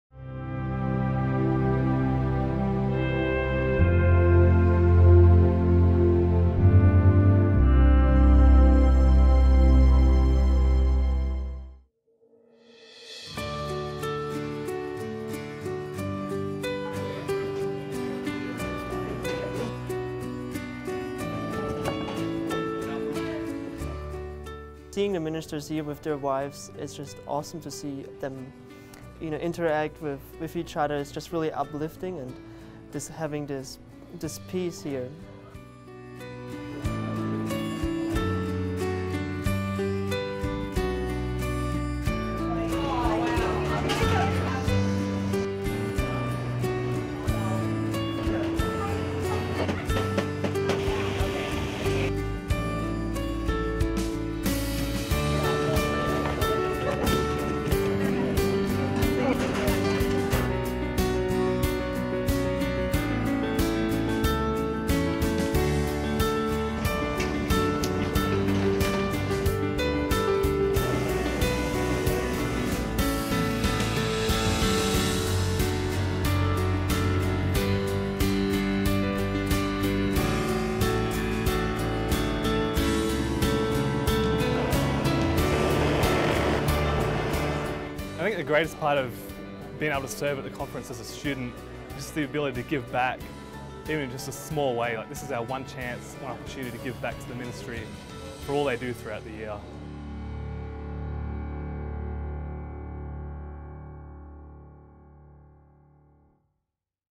HWAC Work-Study Program: Ministerial Conference
EDMOND—Much of the Herbert W. Armstrong College experience revolves around what students spend their energies on outside of class: their responsibilities in the college’s work-study program. At headquarters, students prepare meals, clean buildings, landscape gardens, look after animals, repair appliances, write or fact-check articles, build websites, translate books, assist teachers, edit videos, design publications, snap photos, paint illustrations and assist with social media and online advertising.

The 2015 Philadelphia Church of God Ministerial Conference presented a unique work-study opportunity for 60 students at the headquarters campus. Between December 30, 2015 and January 5, 2016, students broke from their classes and their usual work duties to provide three meals a day for more than 100 visiting ministers and wives as well as the student body. Two shifts of student waitstaff served the ministers and cleaned and set up the hall for each meal, while and expanded custodial crew cleaned and polished Armstrong Auditorium and the John Amos Field House. Take an inside glimpse into this unique aspect of the Armstrong work-study program with this behind-the-scenes video.